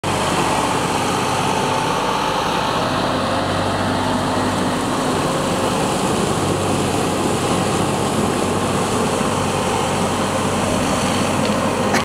ХТЗ Т-150 тащит огромный корень из дерева !!! HTZ T-150 v8 dragging a huge root of the tree
мтз 80 не справился с огромным деревом и пришлось вызвать подкрепления в виде т150 который смог сдвинуть пень и потащил его на свалку !